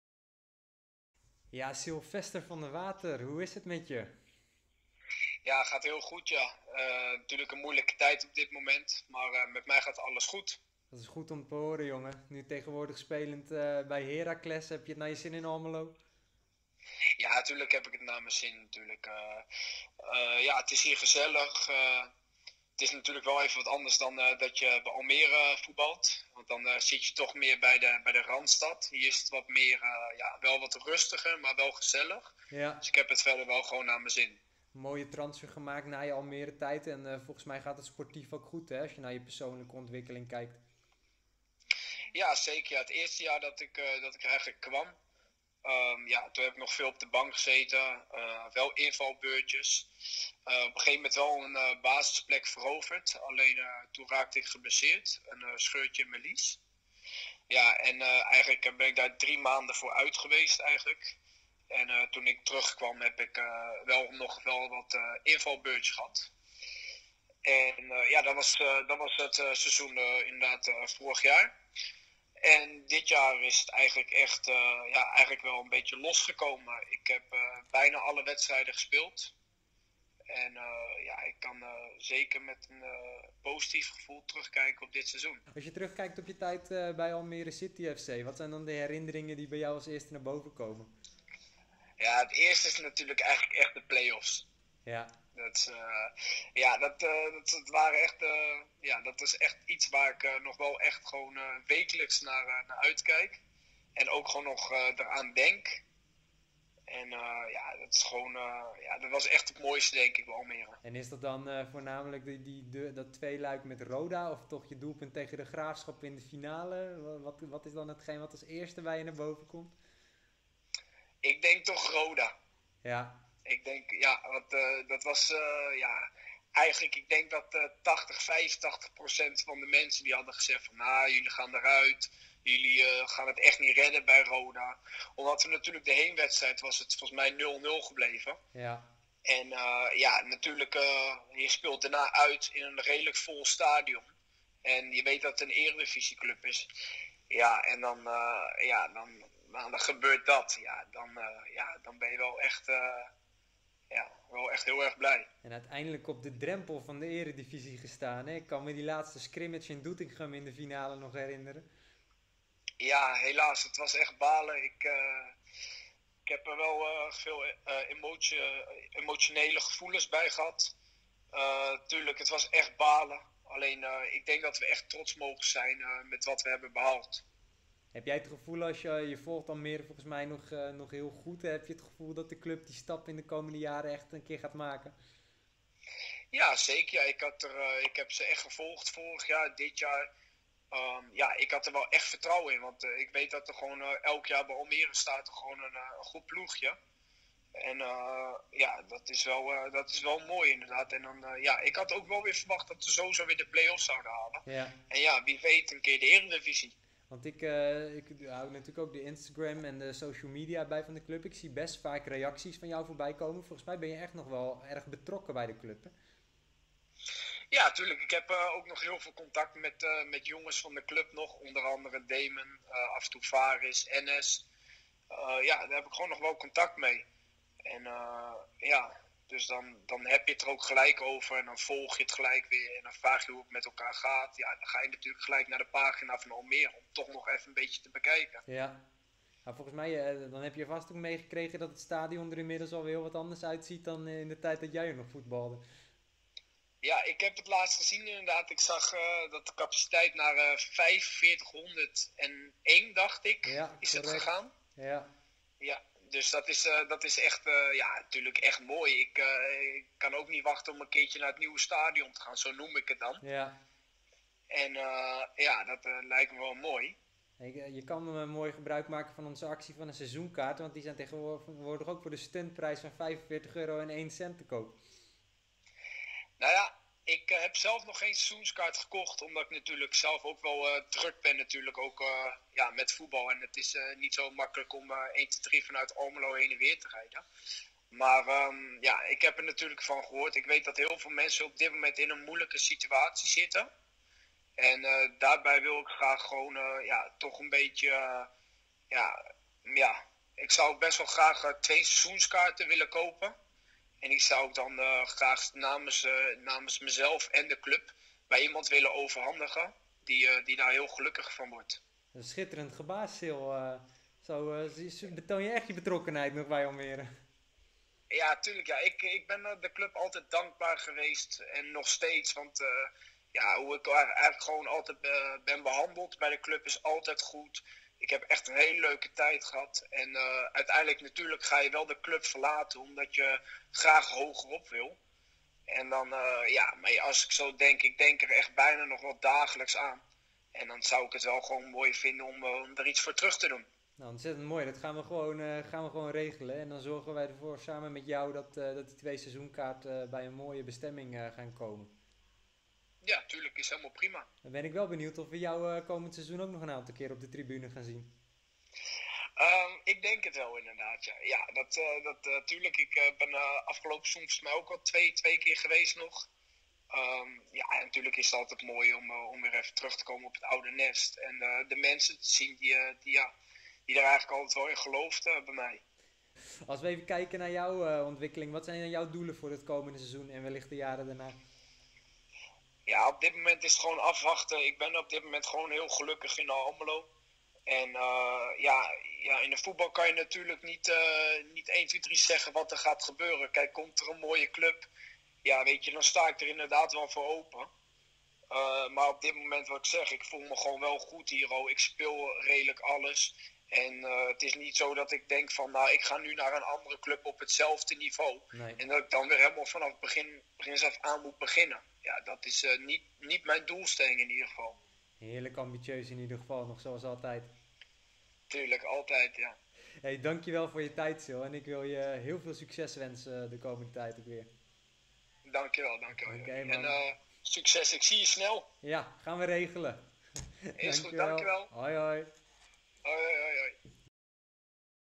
Van der Water: 'Ik denk nog vaak aan de play-offs'
Silvester van der Water blikt terug op zijn tijd bij Almere City FC, vertelt over zijn ontwikkeling bij Heracles Almelo en maakt een mooi gebaar naar de supporters.